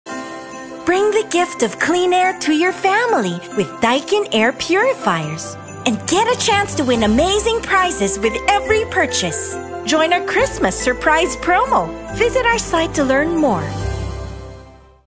Bring the gift of clean and safe air to your home with Daikin Air Purifiers
And get a chance to win amazing prizes when you join our Christmas SurPRIZE Promo!

Get your Daikin Air Purifier to ensure that no unseen viruses, germs, and bacteria stick to the air around you when you enter your home!